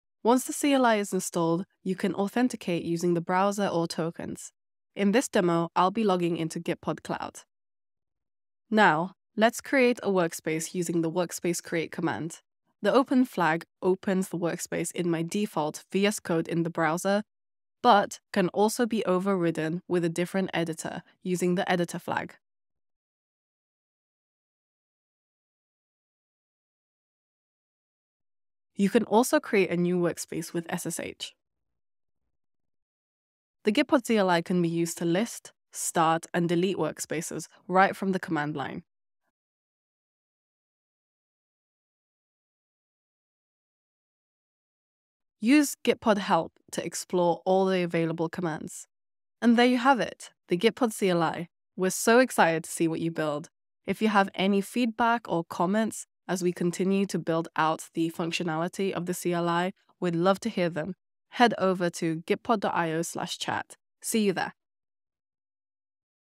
Introducing the Gitpod CLI ⚡️
Gitpod’s developer platform provides on-demand cloud development environments (CDEs) to create software faster and more securely. CDEs are pre-configured with all tools, libraries and dependencies needed to write and review code. They can be easily duplicated and shared across teams.

# Timestamps
0:00 Authentication
0:16 Create a new workspace using `workspace-create`
0:26 `--editor` flag
0:30 Create a new workspace using ssh
0:35 What else can you do with the CLI?
0:46 Use `gitpod help` for all commands
0:53 Thanks for watching!